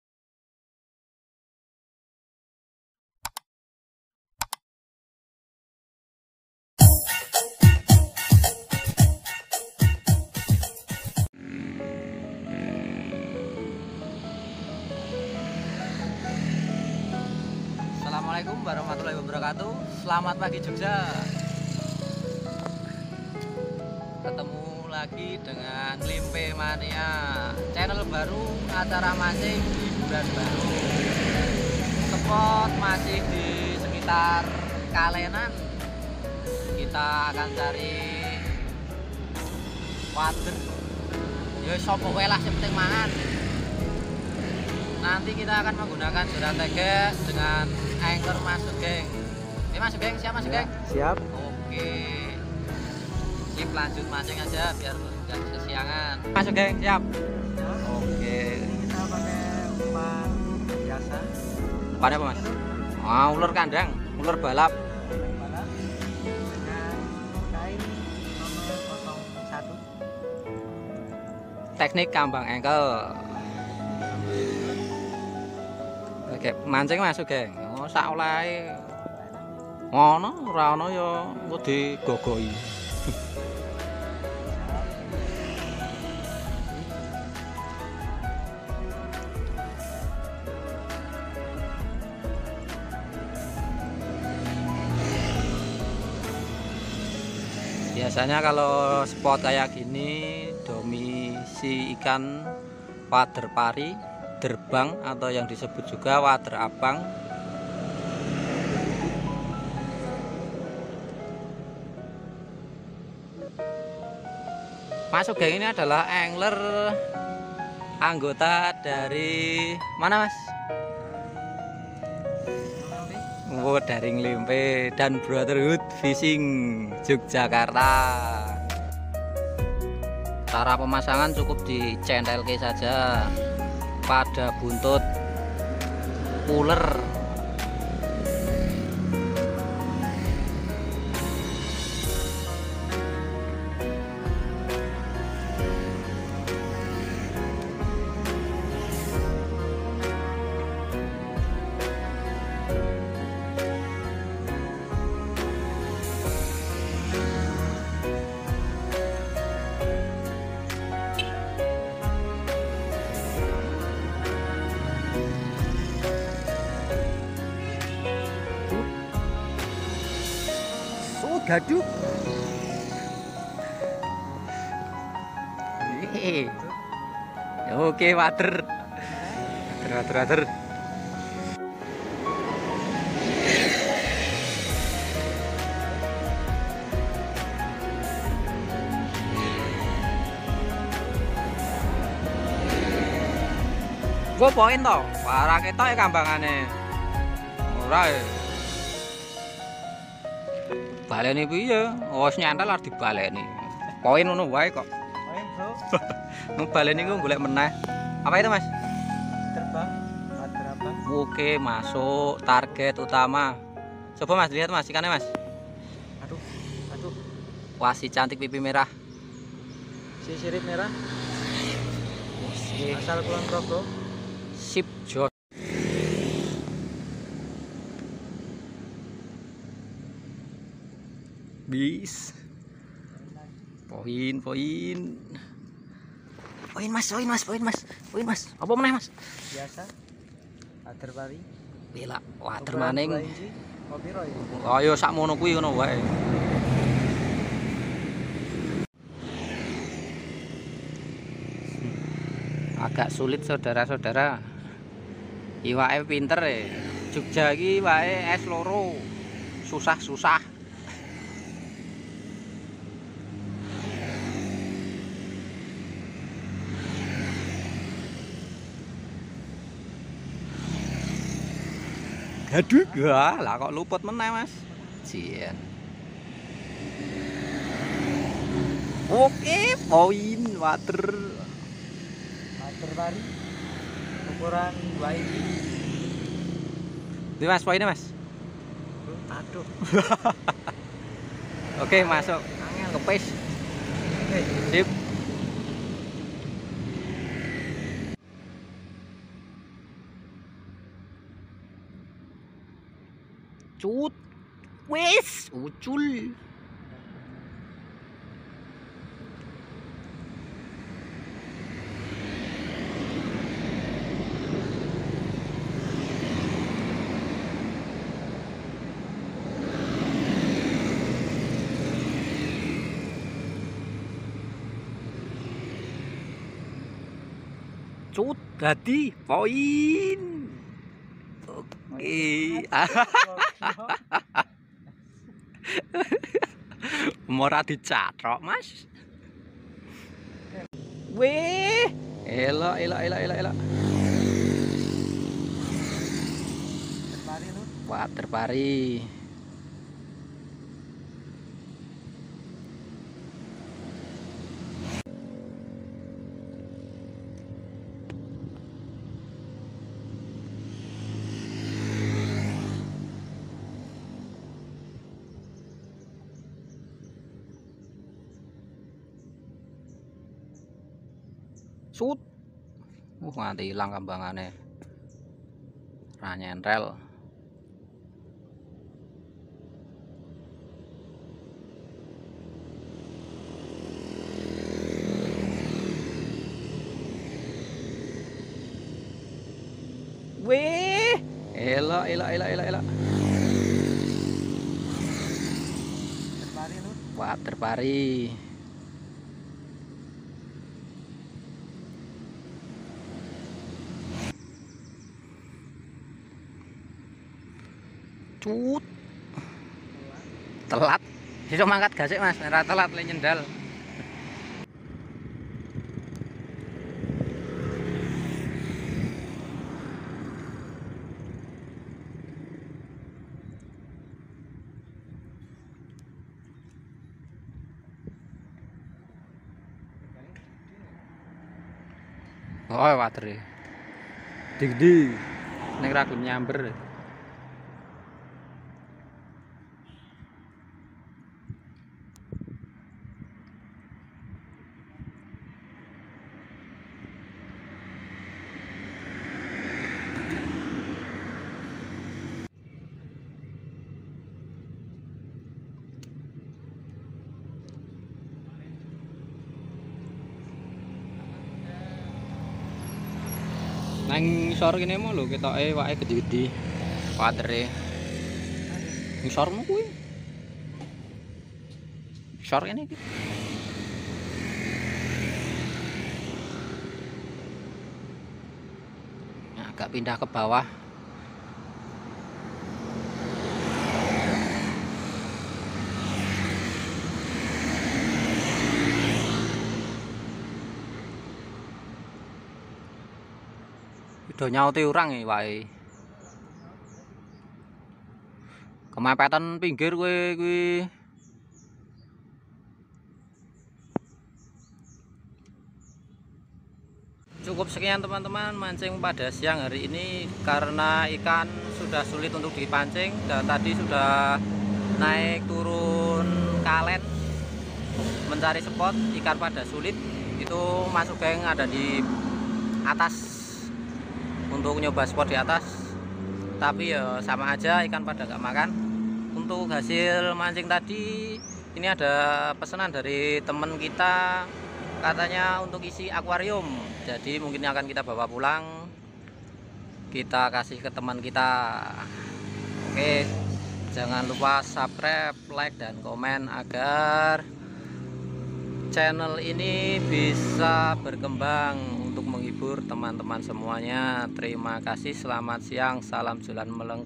Mancing wader//derbang dengan ulat memang top | mancing cantik wader besar babon di sungai terbaru
Ulat (Uler) Umpan Mancing di sepot kalenan (selokan) Ulat (Uler) Umpan Mancing poin ikan cantik si pipi merah Ulat (Uler) Umpan Mancing bikin penasaran.